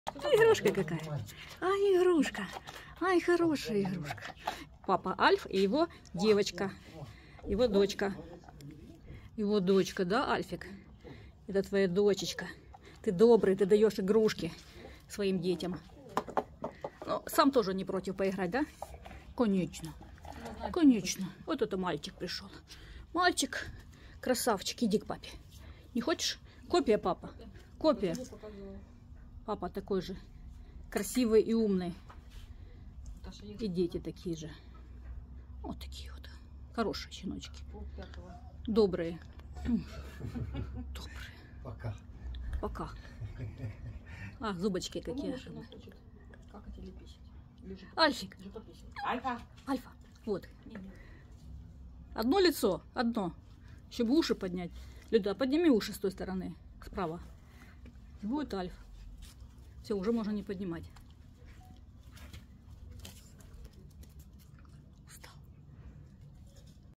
Красавчик Альф и его щенок. Немецкая овчарка. Одесса 2023.
Одесса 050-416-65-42 В продаже щенки Немецкой овчарки от Альфонса фон Тронье. 2023